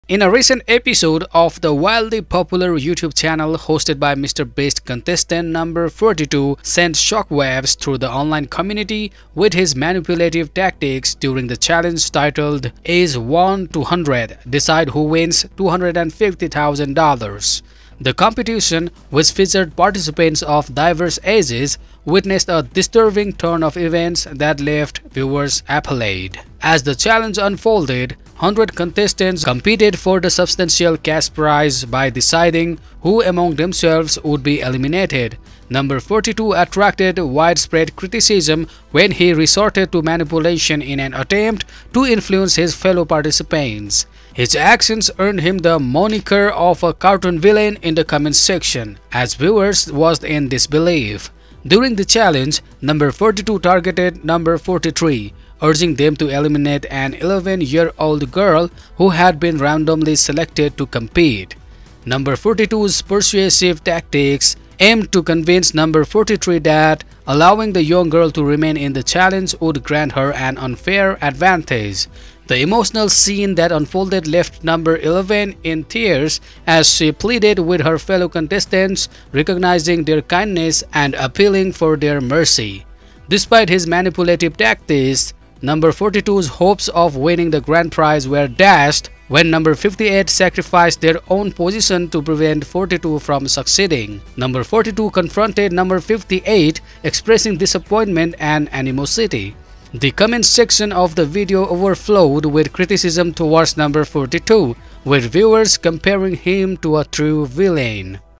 Shocking Manipulation Unleashed: Contestant Sparks Outrage on MrBeast's YouTube Challenge
In a recent episode of the wildly popular YouTube channel hosted by MrBeast contestant number 42 sent shockwaves through the online community with his manipulative tactics during the challenge titled "Ages 1 - 100 Decide Who Wins $250,000." The competition, which featured participants of diverse ages, witnessed a disturbing turn of events that left viewers appalled.
As the challenge unfolded, 100 contestants vied for the substantial cash prize by deciding who among themselves would be eliminated. Number 42 attracted widespread criticism when he resorted to manipulation in an attempt to influence his fellow participants. His actions earned him the moniker of a "cartoon villain" in the comment section, as viewers watched in disbelief.
During the challenge, number 42 targeted number 43, urging them to eliminate an 11-year-old girl who had been randomly selected to compete. Number 42's persuasive tactics aimed to convince number 43 that allowing the young girl to remain in the challenge would grant her an unfair advantage. The emotional scene that unfolded left number 11 in tears as she pleaded with her fellow contestants, recognizing their kindness and appealing for their mercy.
Despite his manipulative tactics, number 42's hopes of winning the grand prize were dashed when number 58 sacrificed their own position to prevent 42 from succeeding. Number 42 confronted number 58, expressing disappointment and animosity
The comments section of the video overflowed with criticism towards number 42, with viewers comparing him to a true villain.